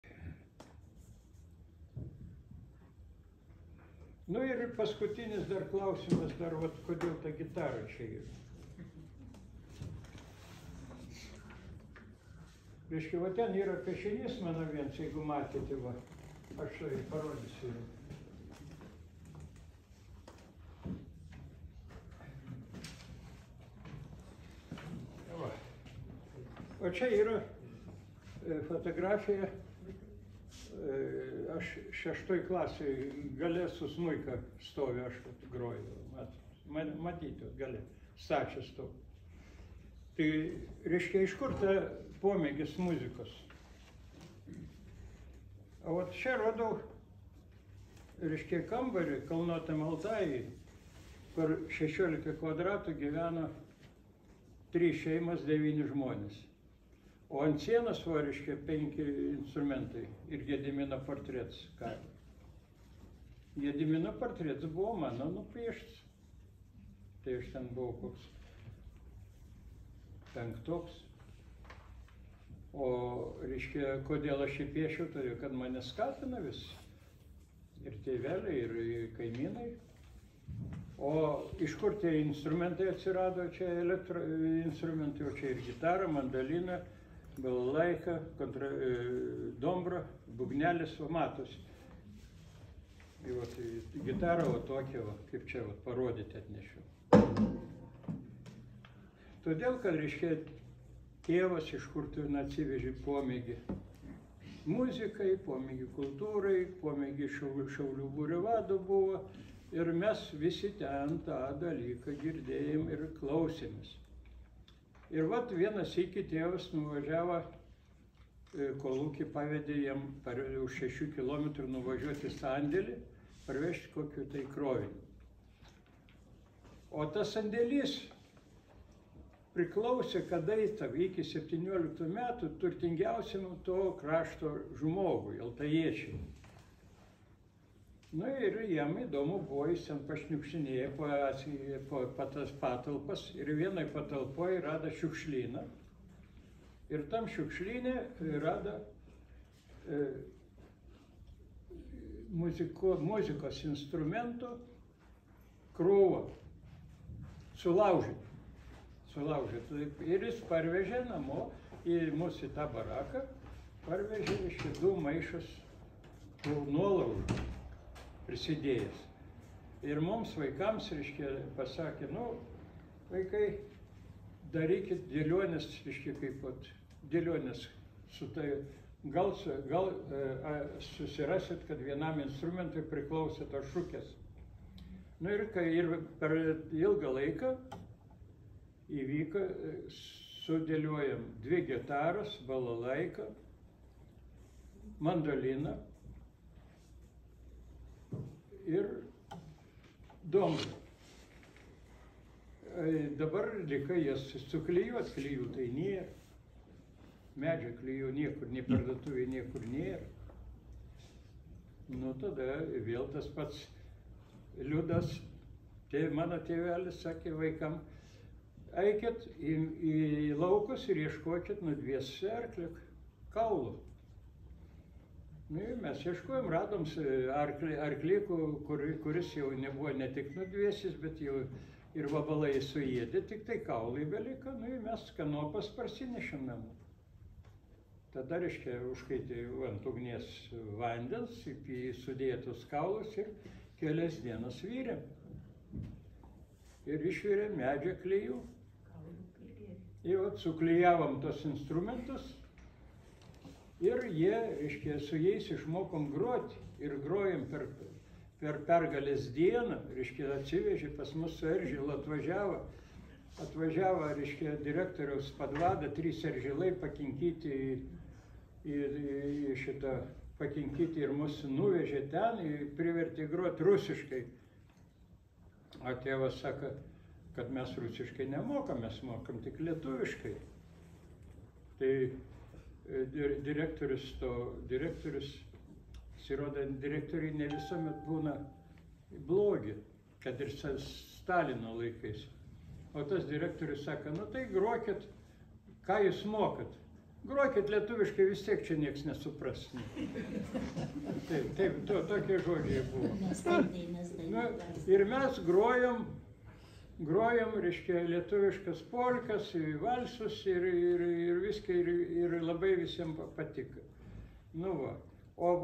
Romualdo Baltucio pasakojimai
Tremtinio Romualdo Baltučio pasakojimai apie gyvenimą Altajuje.